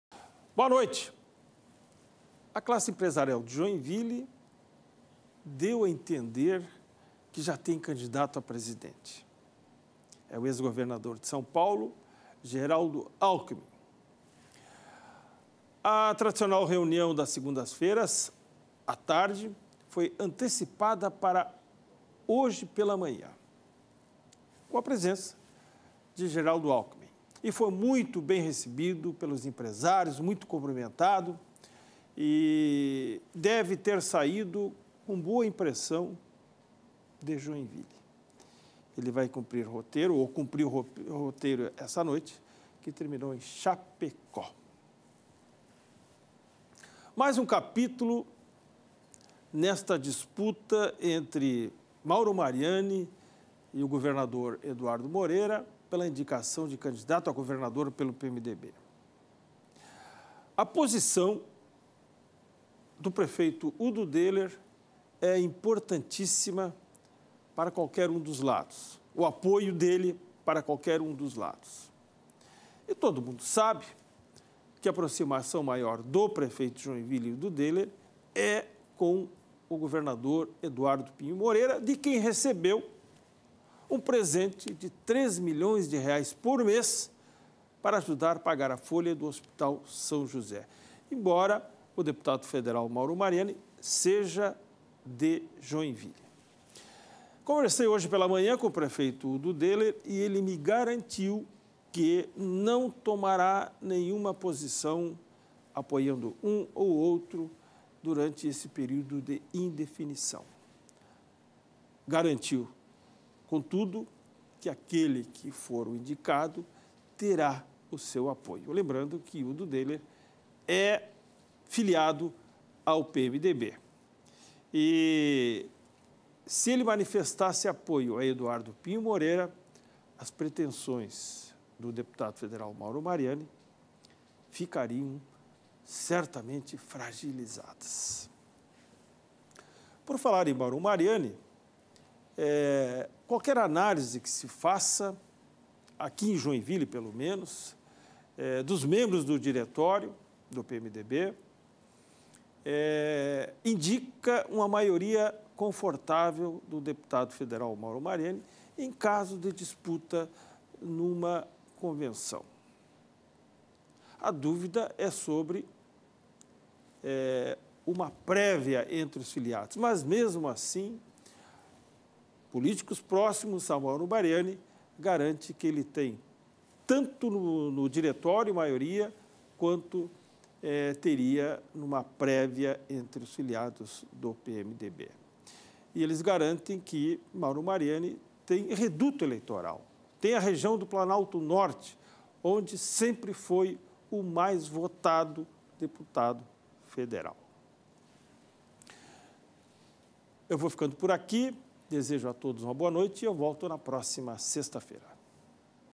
Luiz Veríssimo: classe empresarial de Joinville já tem candidato a presidente
A classe empresarial de Joinville deu a entender que já tem candidato a presidente, é o ex-governador de São Paulo Geraldo Alckimin. A tradicional reunião da segundas-feira a tarde foi antecipada para sexta-feira (8), pela manhã, com a presença de Geraldo, que foi muito bem recebido e cumprimentado pelos empresários.